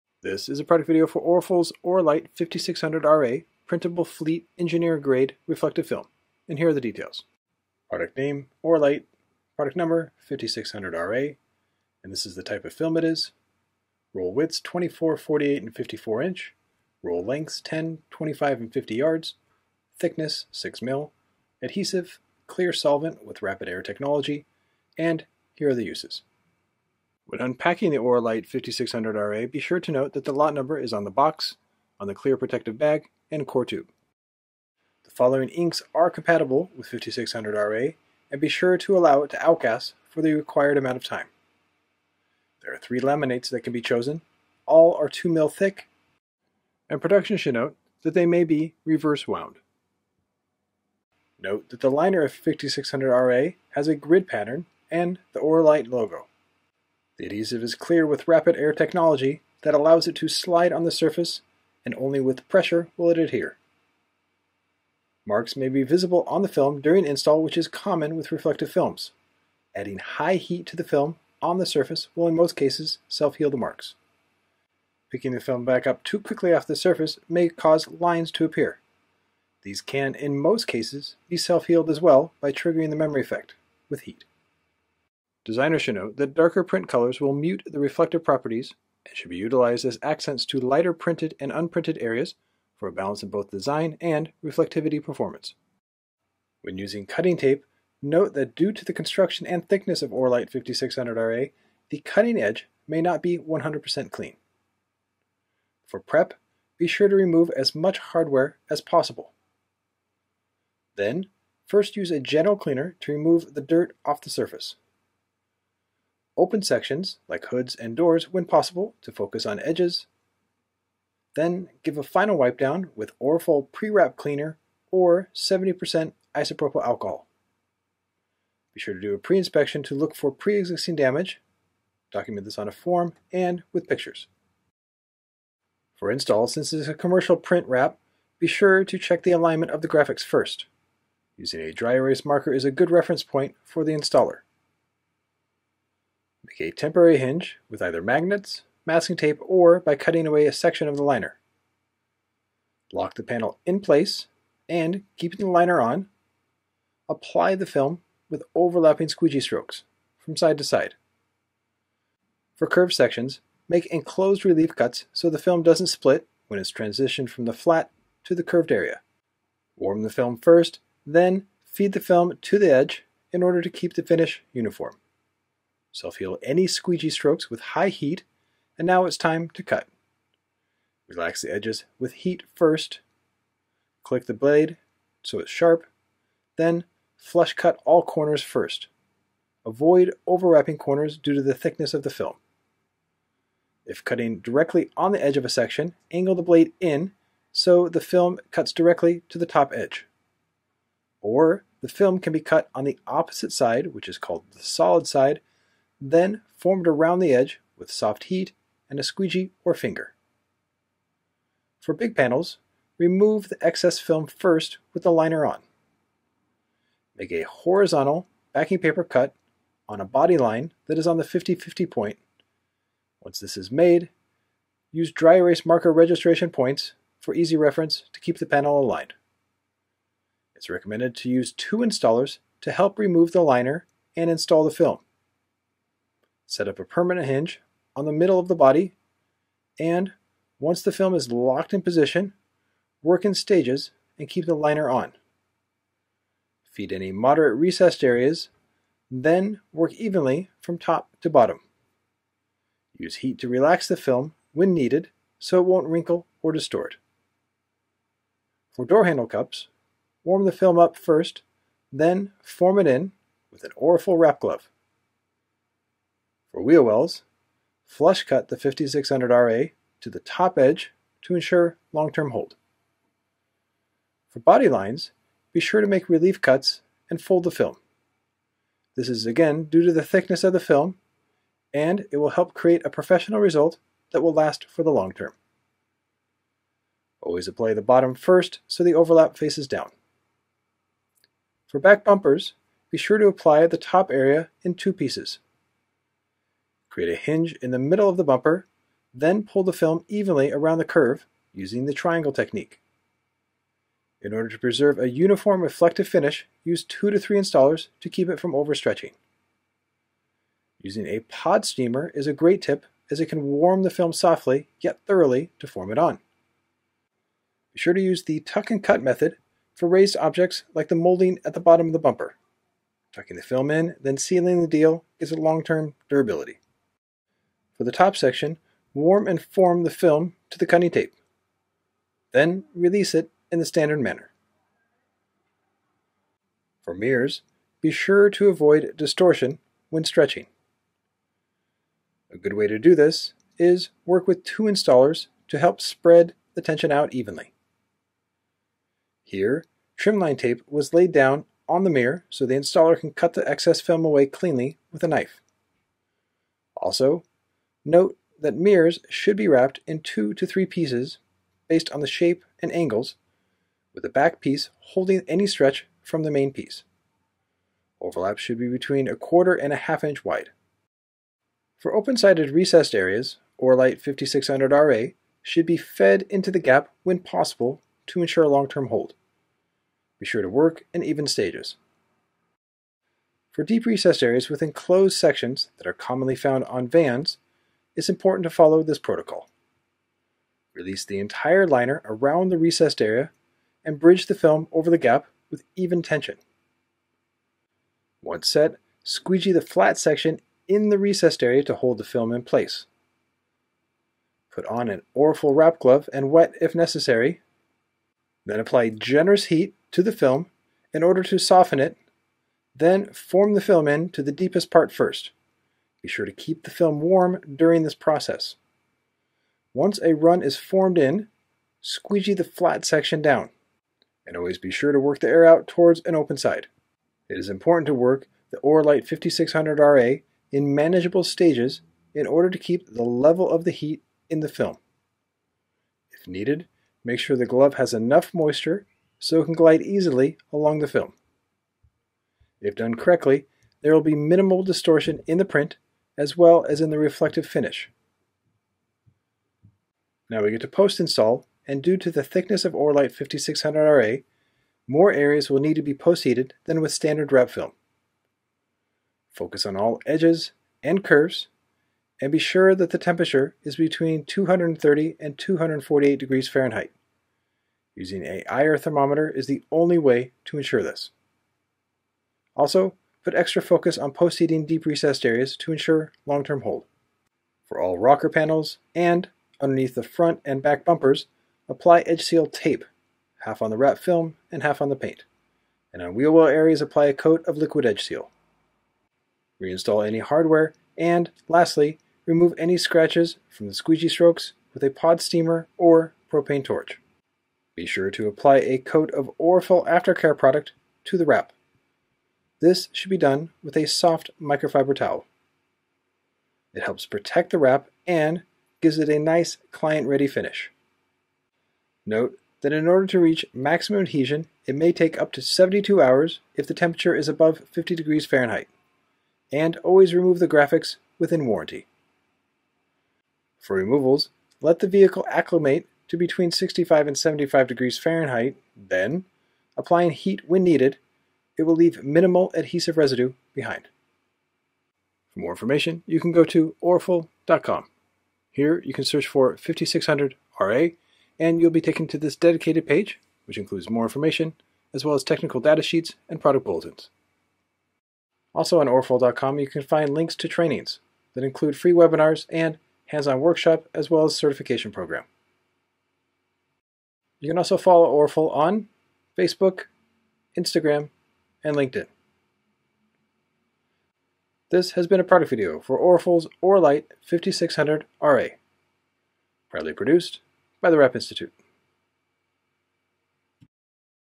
ORALITE 5600RA Fleet Engineer Grade Reflective Wrap Film - ORAFOL Americas
In this video, Justin Pate from @WrapInstitute, provides detailed instructions for wrap installers to utilize while installing ORALITE 5600RA Fleet Engineer Grade Reflective Film to a vehicle.

Surface Preparation
Cutting Tape Strategy
Panel Alignment
Flat Area, Recessed, and Compound Curve Installation Methods
How to Effectively Trim & Cut the Material
Post-Heating Recommendations
Easy Removal Techniques

ORALITE 5600RA Fleet Engineer Grade Reflective Film is Purpose Built for the Automotive Application!
Full Wraps, Partial Wraps, Spot Graphics, Striping, & Lettering – We’ve got you covered!